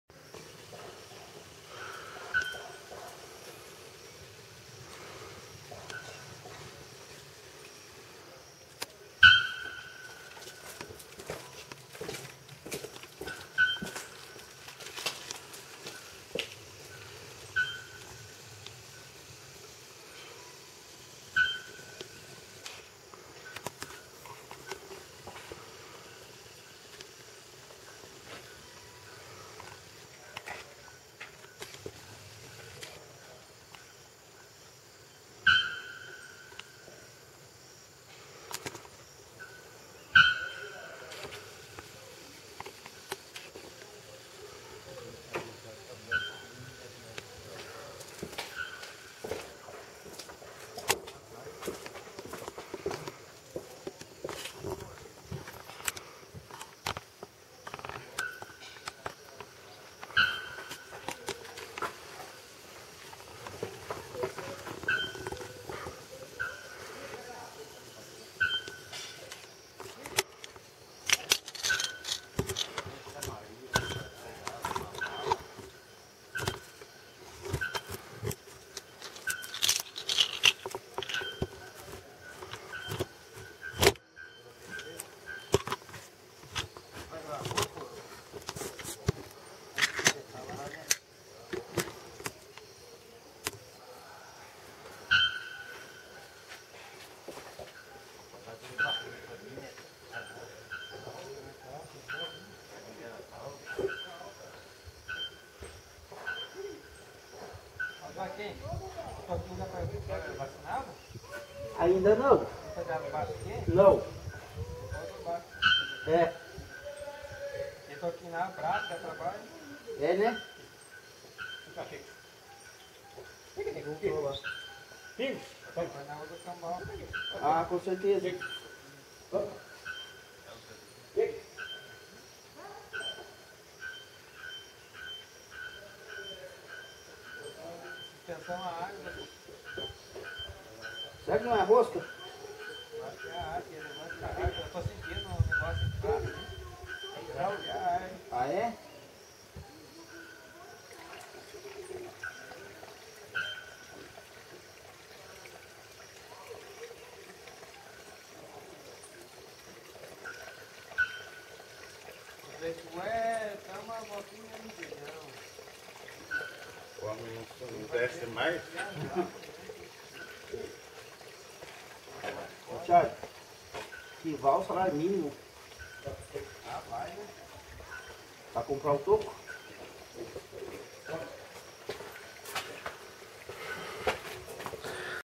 Canto da Araponga
O barulho que parece ser de um martelo batendo é o canto da Araponga, tentei dar um zoom aonde era provavelmente o ninho dele.

Peço desculpas pelas as conversas que atrapalham um pouco o áudio.